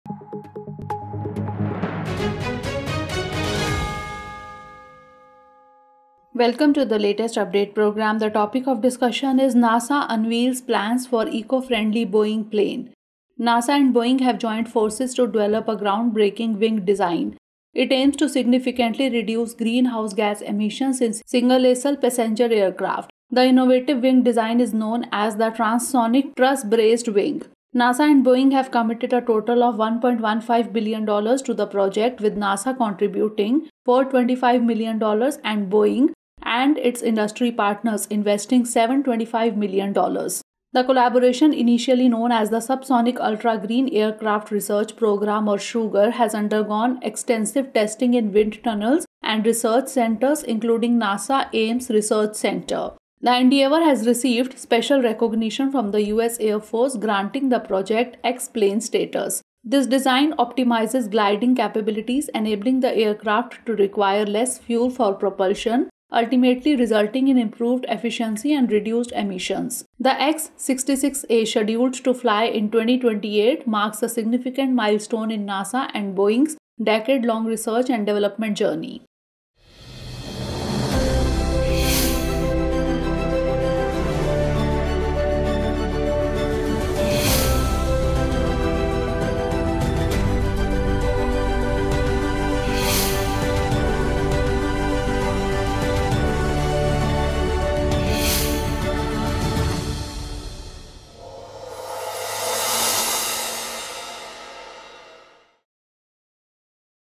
NASA Unveils Plans For Eco-friendly Boeing Plane: Latest update | Drishti IAS English
NASA and Boeing have joined forces to develop a groundbreaking wing design. It aims to significantly reduce greenhouse gas emissions in single-aisle passenger aircraft. The innovative wing design is known as the Transonic Truss-Braced Wing.